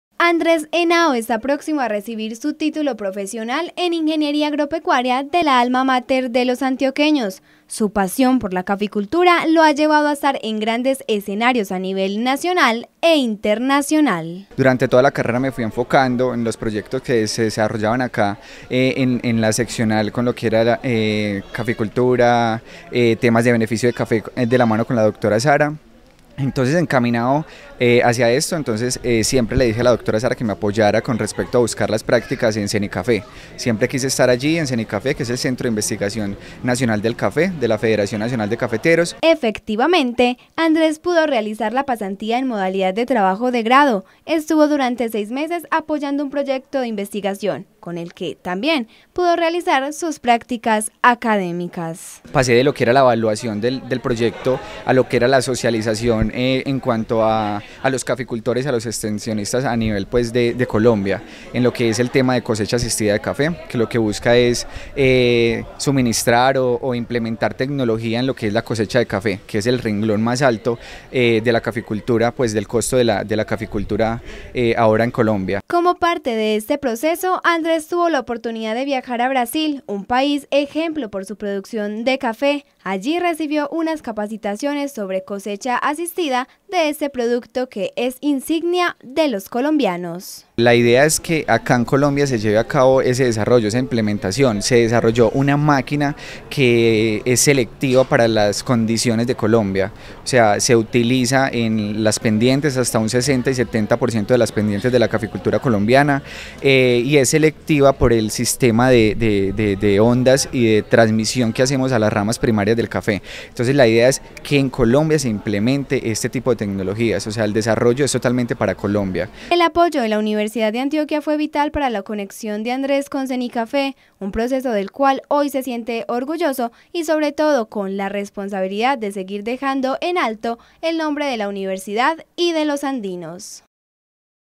ANDRÉS HENAO, UN ESTUDIANTE EJEMPLAR DE LA UDEA
Los logros de los estudiantes de la Seccional Suroeste de la Universidad de Antioquia, son un orgullo para toda la subregión, por eso en UNS El Informativo, resaltamos los procesos académicos de estos estudiantes.